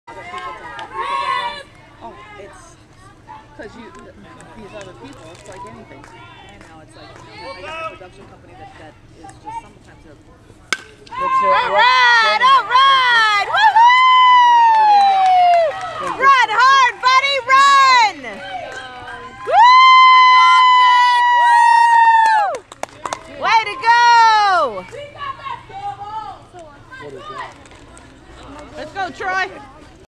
Jake rips a double!
Made a bet with Jake that he had to hit a double to stay in the game pitching!